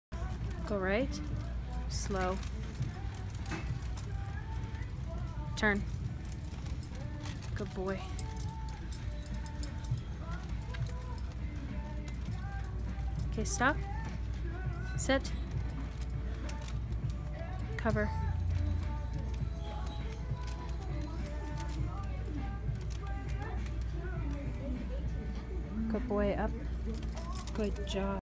Service Dog working in store
This is another video from working in the store. He has to know directional cues because he works with mobility forward momentum, so he gets there before I do. He is also holding an item for me.